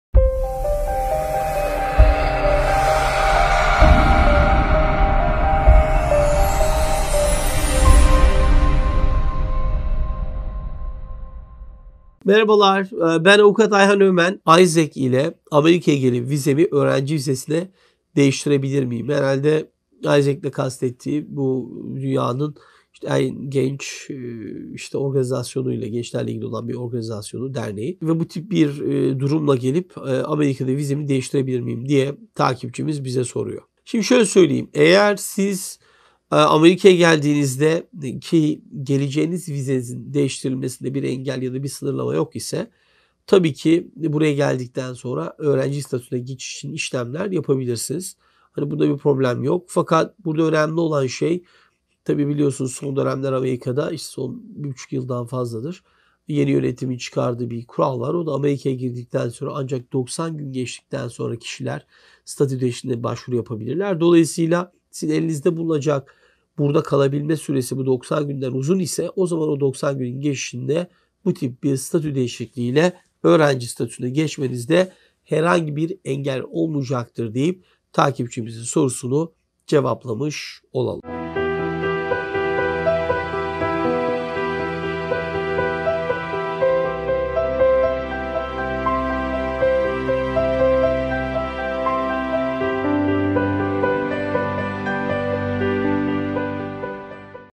AIESEC PROGRAMIYLA AMERİKA’YA GELENLER F1 ÖĞRENCİ VİZESİNE NASIL GEÇER ?
Soru: Aiesec (AIESEC is the world's largest youth-run organization) ile Amerika’ya gelip vizemi öğrenci vizesi ile değiştirebilir miyim ?